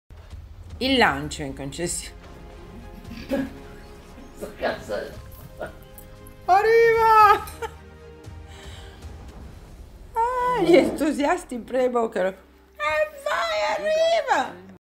Quando si lancia una macchina sul mercato – Electric Motor News e Motor News
Le immagini esclusive del lancio di una macchina, raccontate da Lilia Marcucci… ogni tanto scherziamo in Electric Motor News e in Motor News, con il solo intento di alleggerire la vita e strappare una risata…